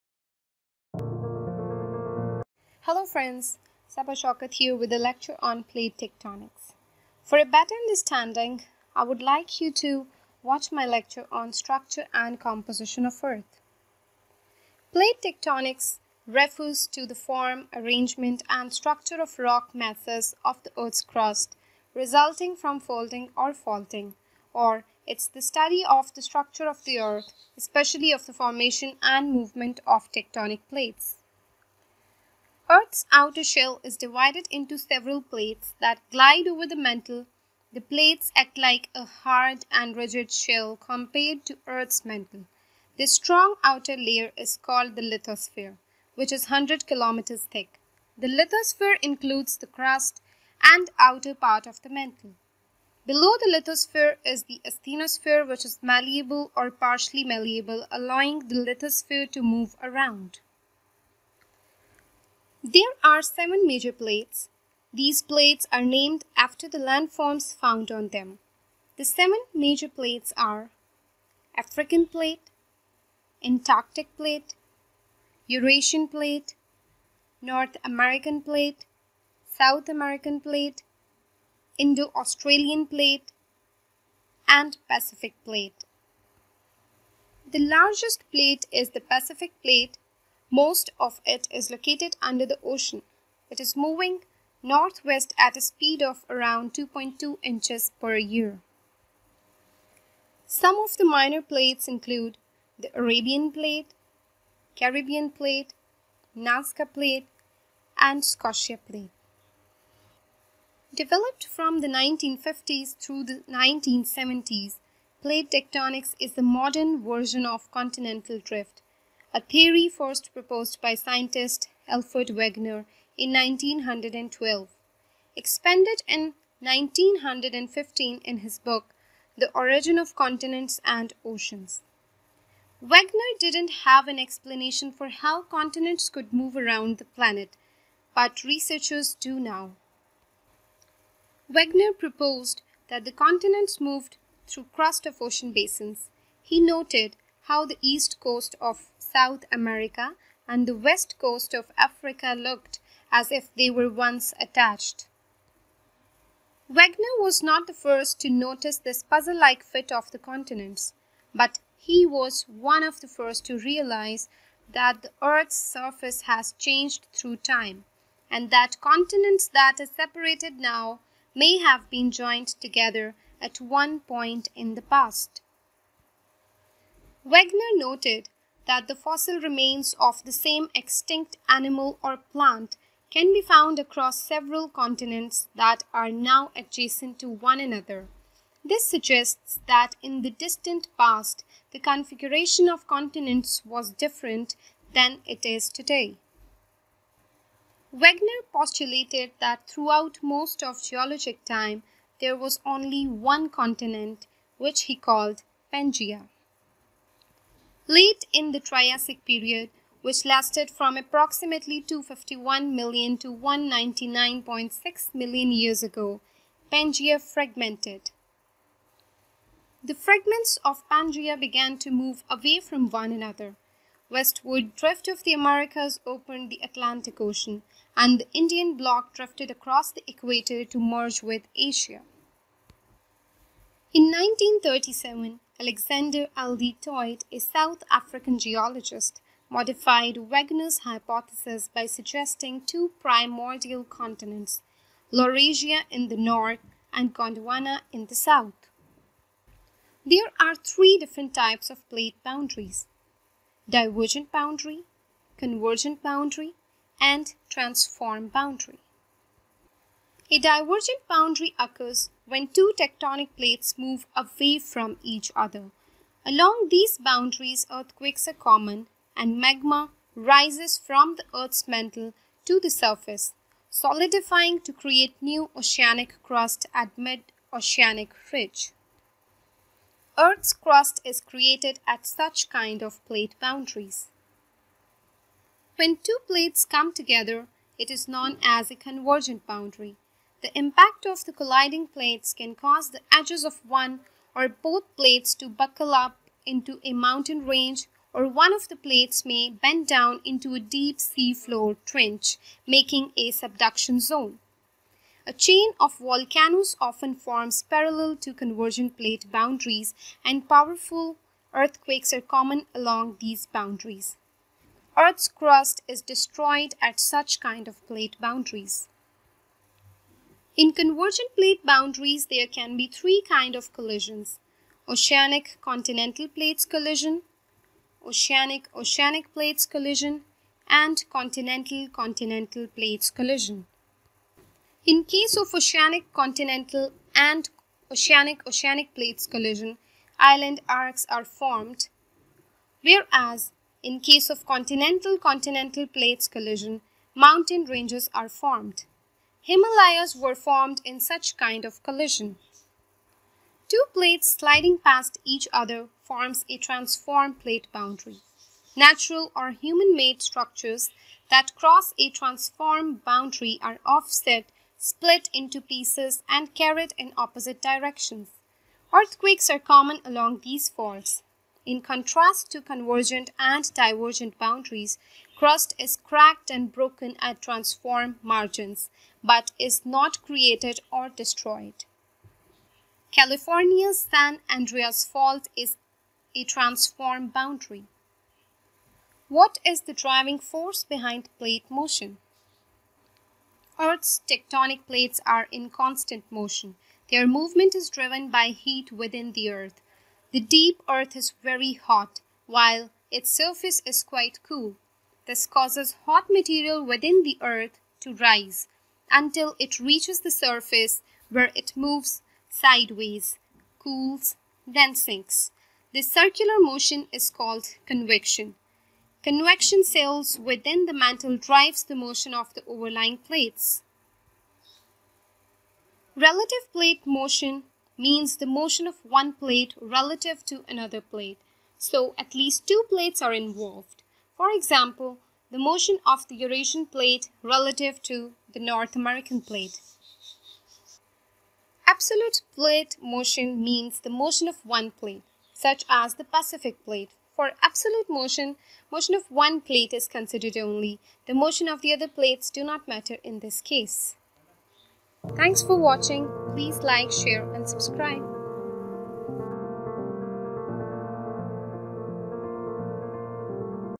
Plate Tectonics Lecture | Continental Drift Theory | Plate Boundaries | Lecture by Saba Shoukat
Lecture by Environmentalist of Pakistan on plate tectonics, historical perspective of continental drift theory, different types of plate boundaries and the driving force behind plate motion. Best lecture for CSS aspirants preparing Environmental Sciences.